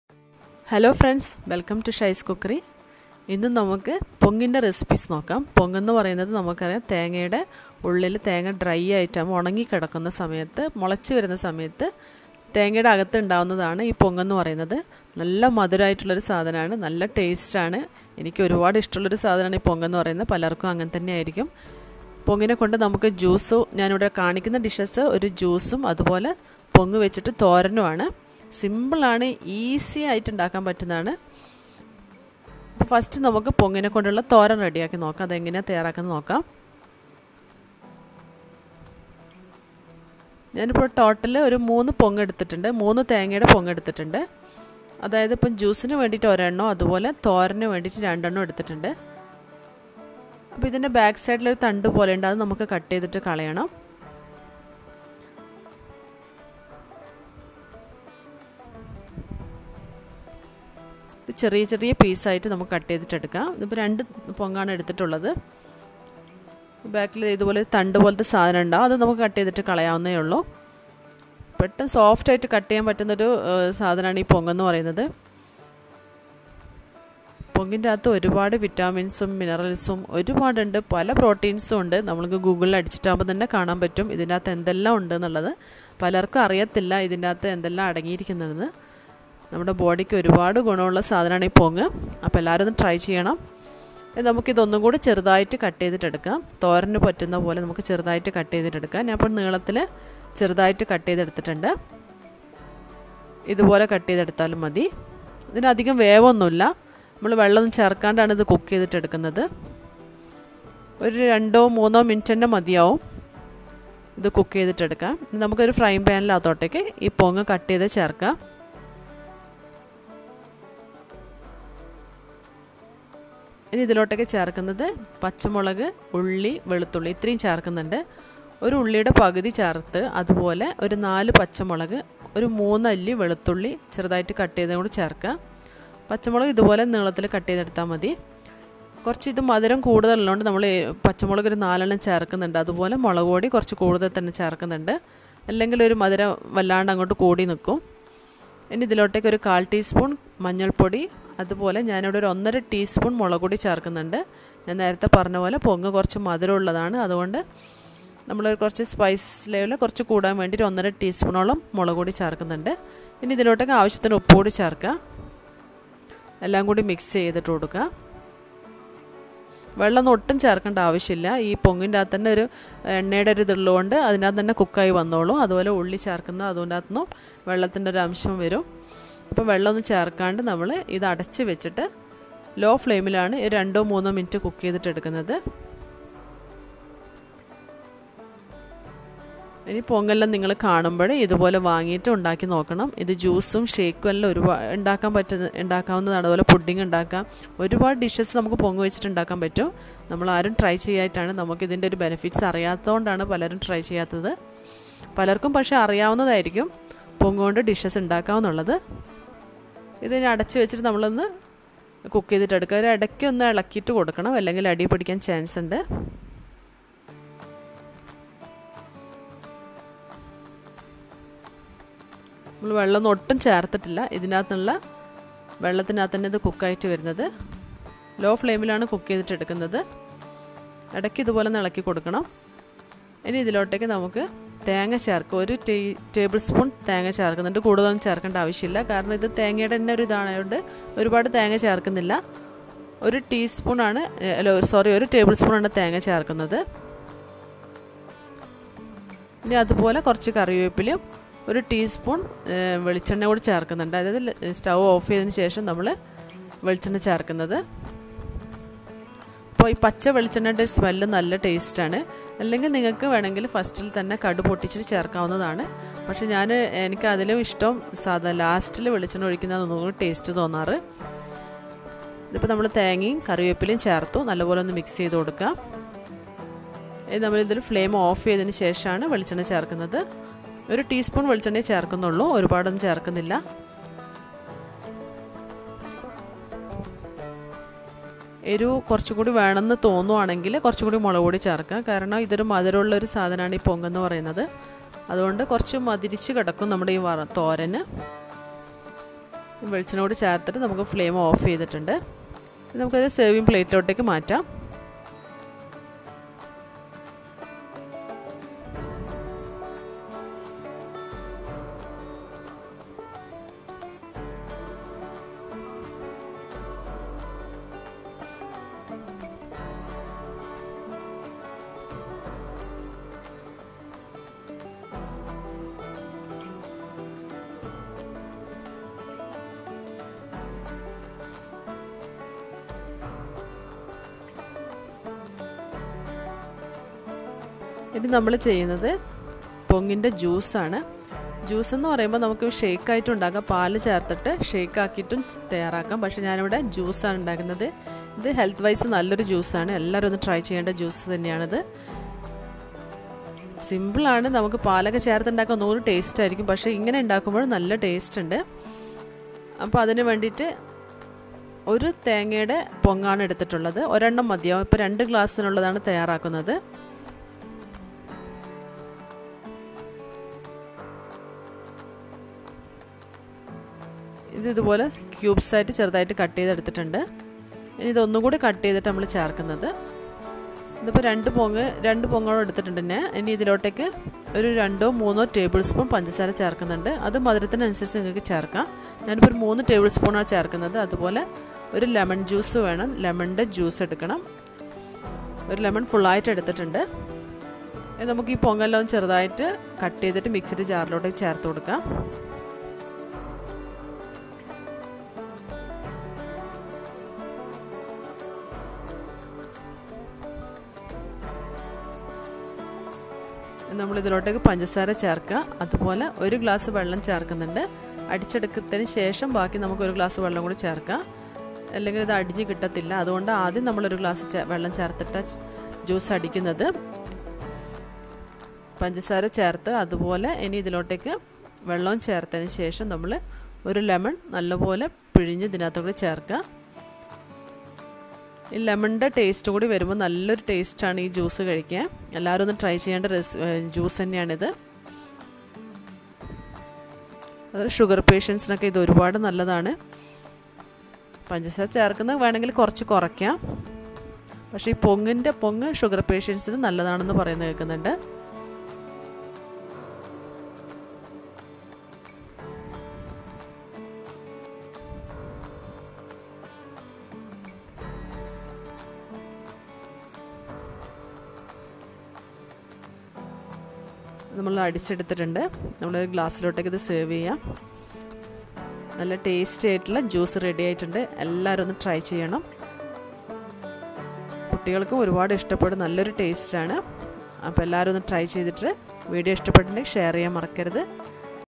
||പൊങ്ങ് ഉപയോഗിച്ച് പോഷകപ്രദമായ ജ്യൂസ്, തോരൻ ഉണ്ടാക്കാം, രുചിയിൽ അതികേമൻ||Coconut Apple recipe
Coconut apple recipes
Juice and thorran
Two healthy recipes